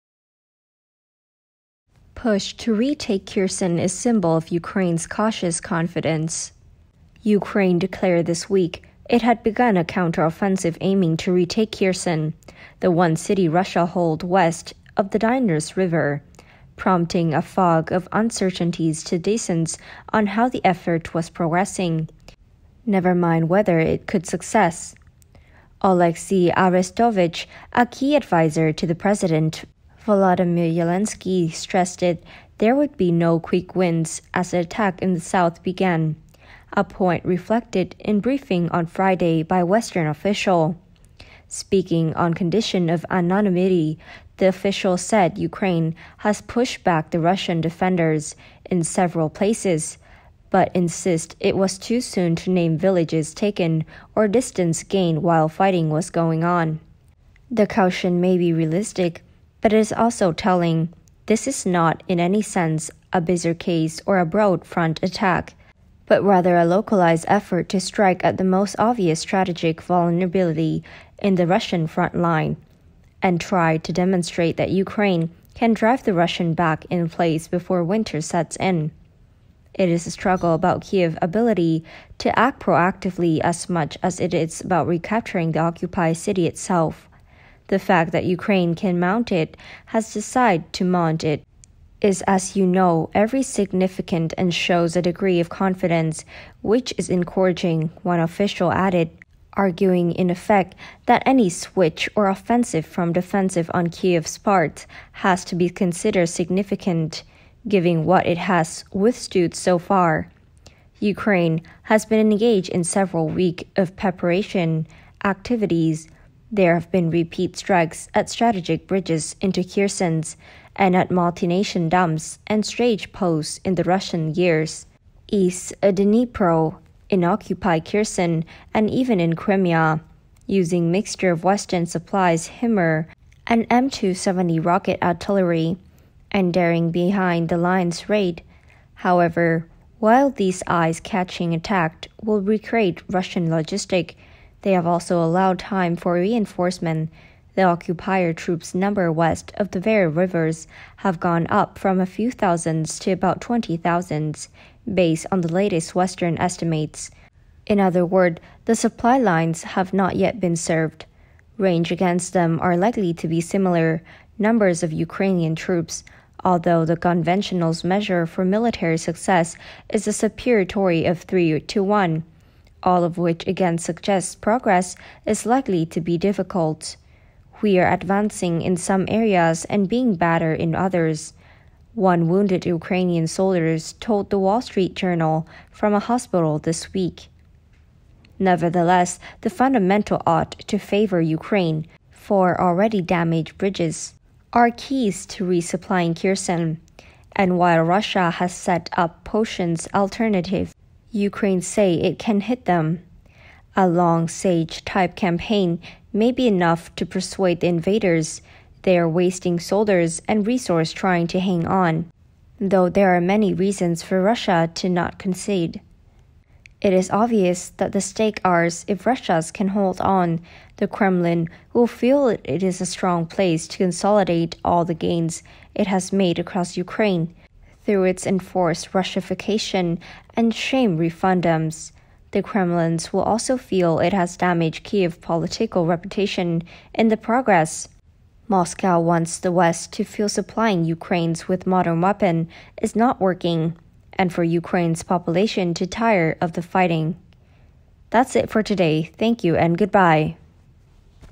Push to retake Kherson symbol of Ukraine | BBS News
Ukraine declared this week it had begun a counteroffensive aiming to retake Kherson – the one city Russia holds west of the Dnieper River – prompting a fog of uncertainty to descend on how the effort was progressing, never mind whether it would succeed.